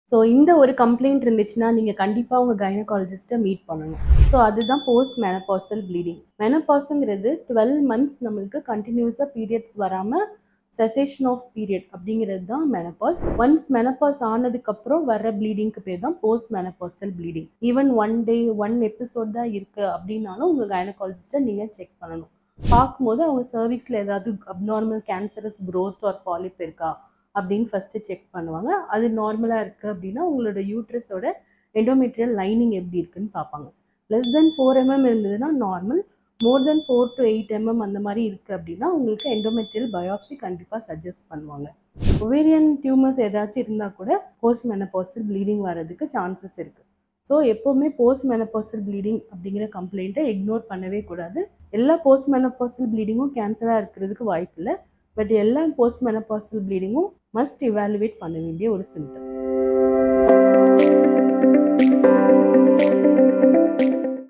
What is Postmenopausal Bleeding | Dr Ramapraba Gnecologist
- Postmenopausal bleeding refers to any vaginal bleeding that occurs after a woman has gone through menopause, which is the permanent cessation of menstrual periods.

- Menopause typically occurs around the age of 50, and postmenopausal bleeding is considered abnormal. While it may result from benign causes like hormonal fluctuations or vaginal atrophy, it could also indicate more serious conditions such as endometrial hyperplasia or cancer. Any instance of postmenopausal bleeding should be promptly evaluated by a healthcare professional to determine its cause and initiate appropriate treatment.

- Regular medical check-ups and open communication with a healthcare provider are crucial for women's health during and after menopause.

Follow @varamm_healthcare_pvt_ltd